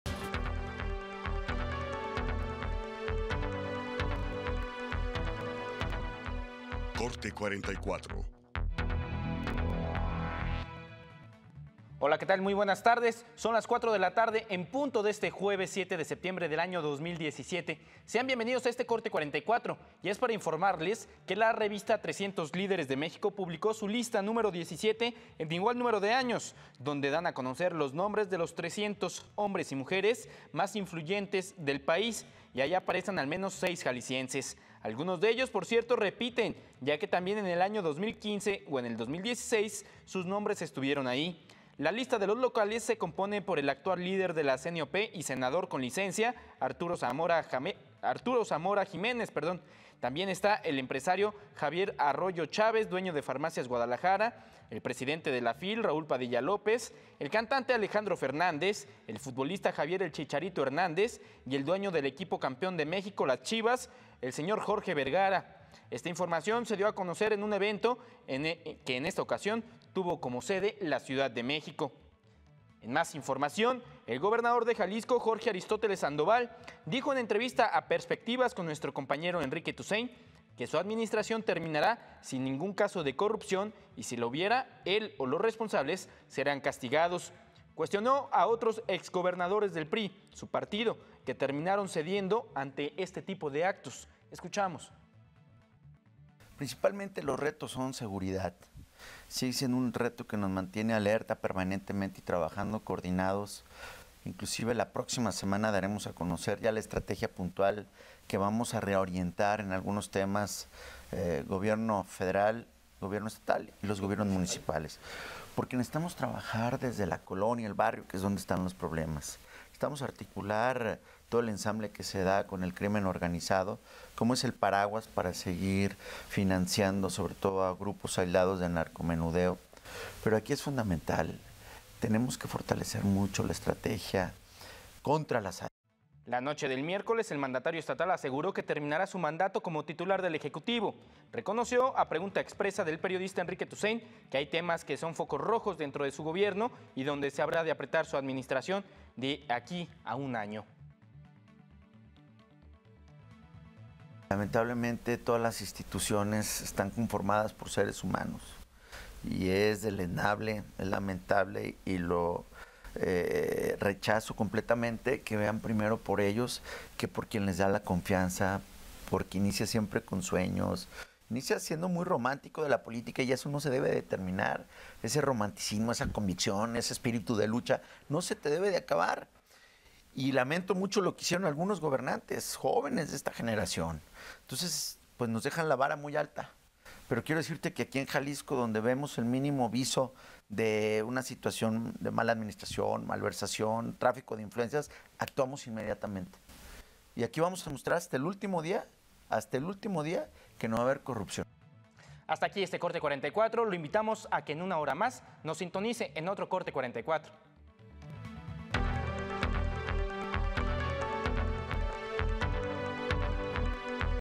Corte 44 | Aparecen al menos seis jaliscienses en la revista 300 líderes de México
⚡ La revista 300 líderes de México publicó su lista número 17 -en igual número de años- donde da a conocer los nombres de los 300 hombres y mujeres más influyentes del país y ahí aparecen al menos seis jaliscienses.

⚡ El gobernador de Jalisco, Jorge Aristóteles Sandoval, dijo en entrevista a Perspectivas, con nuestro compañero Enrique Toussaint, que su administración terminará sin ningún caso de corrupción y si lo hubiera, él o los responsables serán castigados.